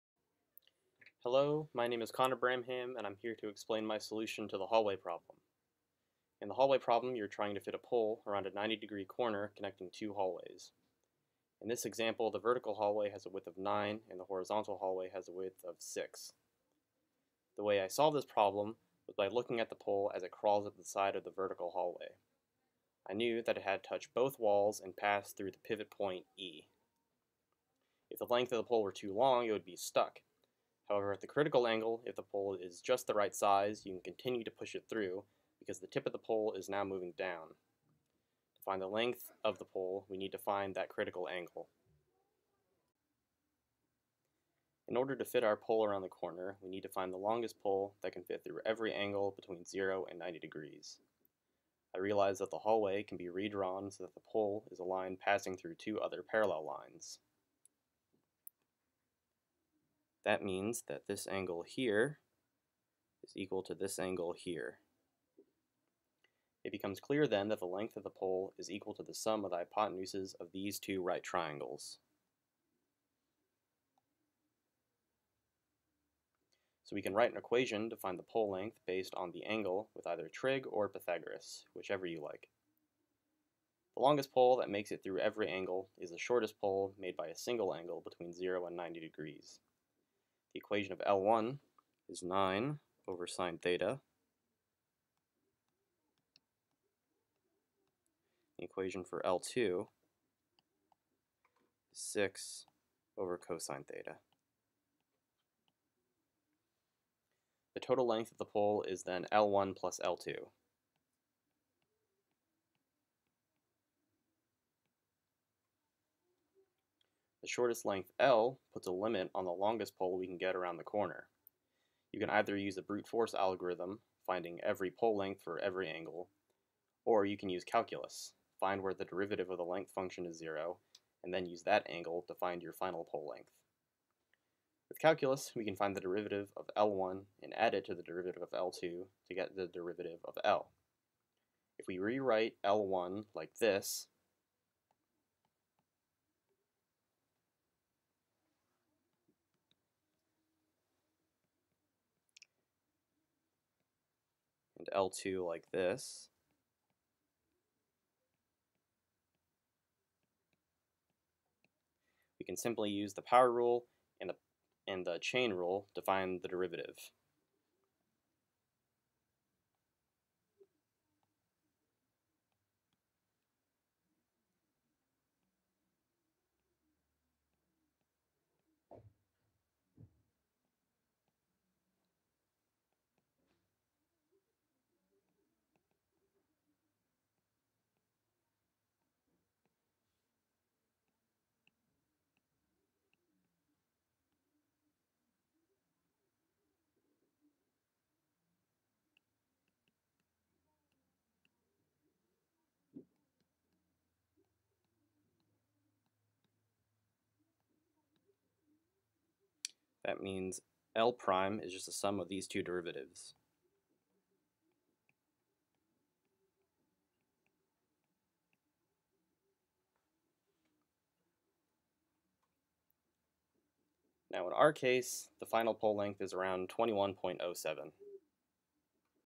Longest Pole that Fits around the corner of two hallways
Connor Bramham explains the solution of a well-known problem found in calculus texts. A ninety-degree angle is formed by the intersection of two hallways. Carry the pole down a 9ft wide hallway and turn 90 degrees into a 6ft wide hallway. What is the longest pole that makes the corner while keeping the pole in the horizontal plane.
The problem can be solved by calculating all poles in either Excel or Matlab, or can be solved using calculus.